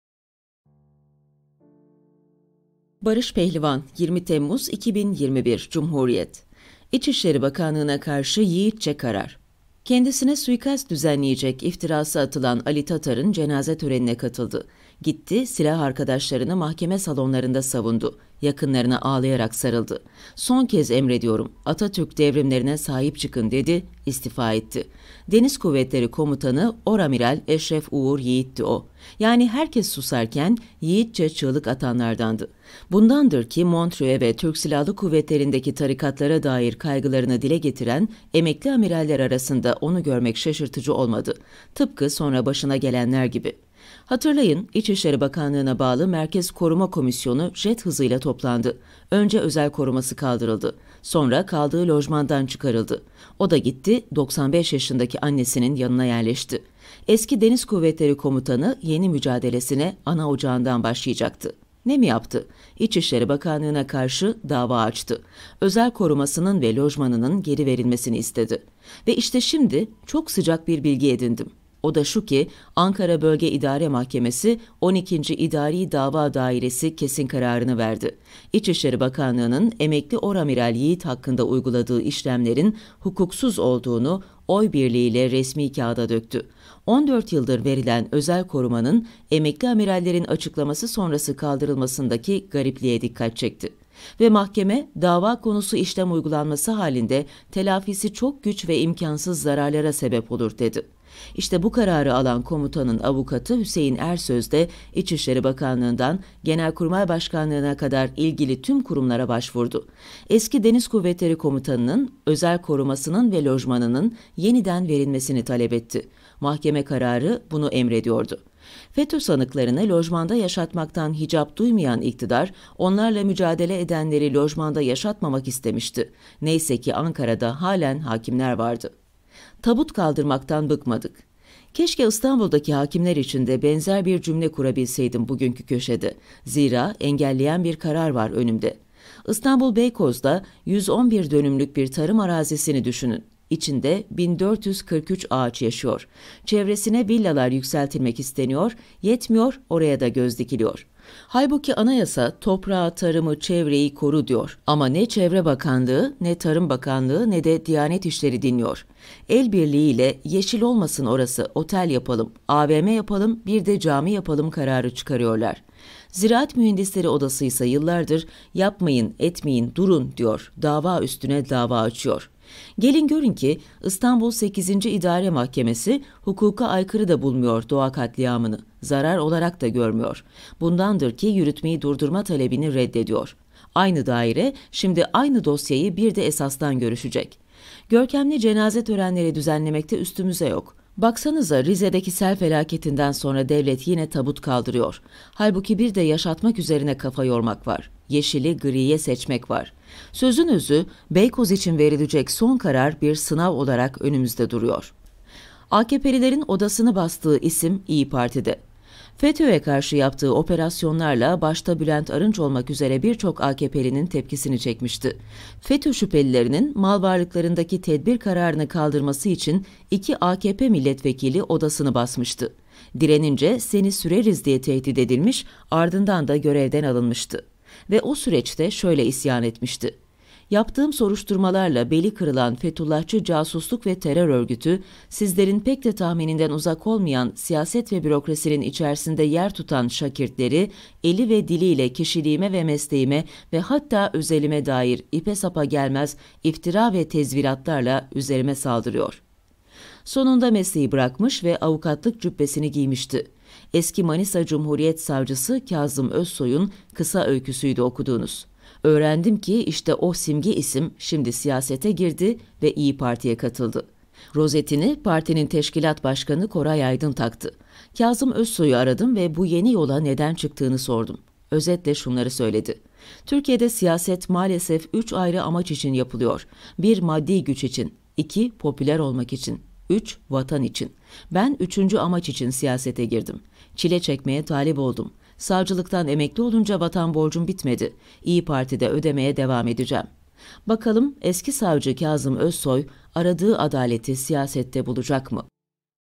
BARIŞ PEHLİVAN "İçişleri Bakanlığı'na Karşı Yiğitçe Karar" 20 Temmuz 2021 Sesli Köşe Yazısı Dinle
Şu anda, ' Barış Pehlivan 'ın 20 Temmuz 2021 tarihinde ' Cumhuriyet 'te yayınlanan ' köşe yazısı 'nı dinliyorsunuz.